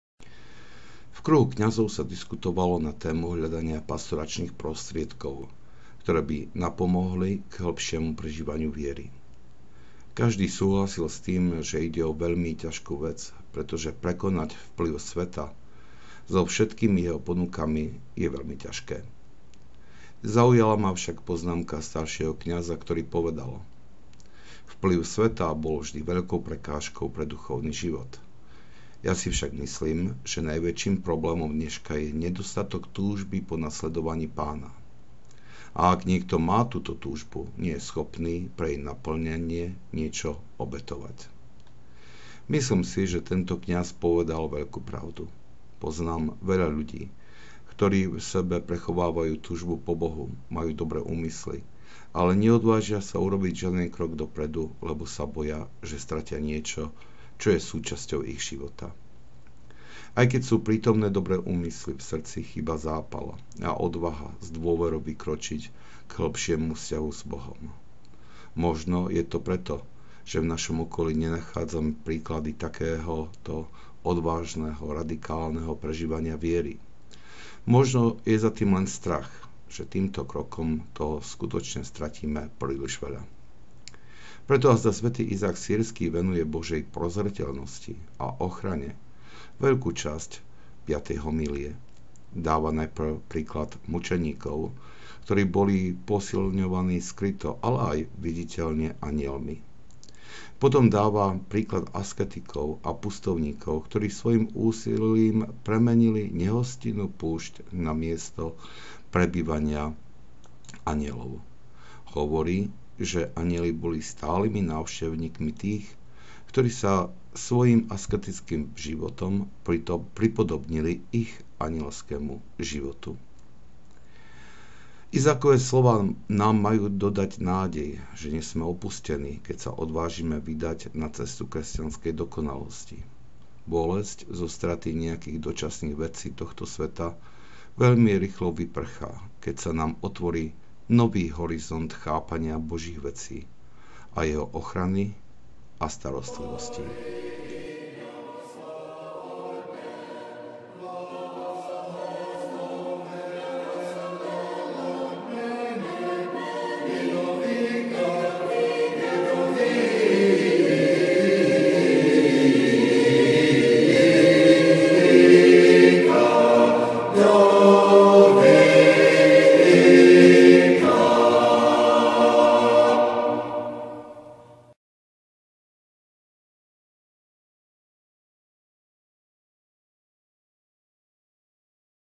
Otvoriť sa pre nové horizonty
Homília 5 – Reflexie nad Asketickými homíliami svätého Izáka Sýrskeho